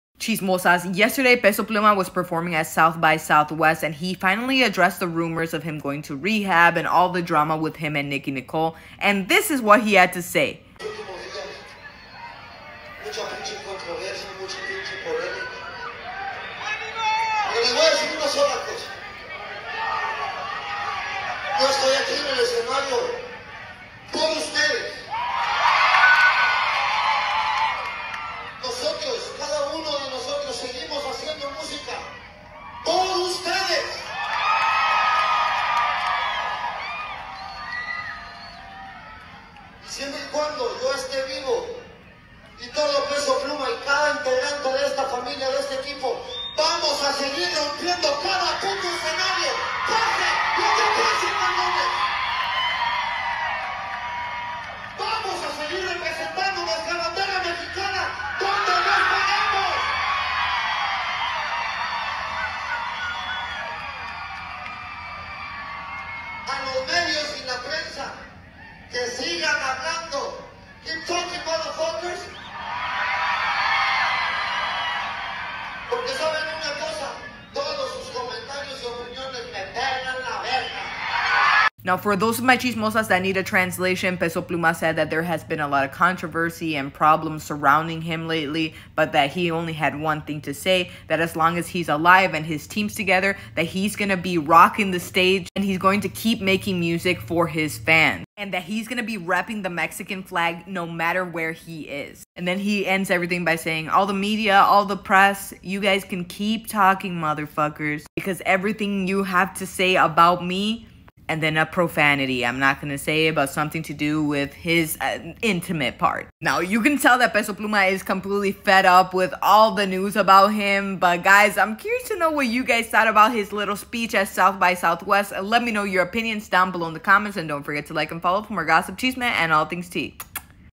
Peso Pluma addresses all the rehab rumors at sxsw
Peso Pluma addresses all the rehab rumors at sxsw sxsw sxsw2024 pesopluma pesopluma💯👹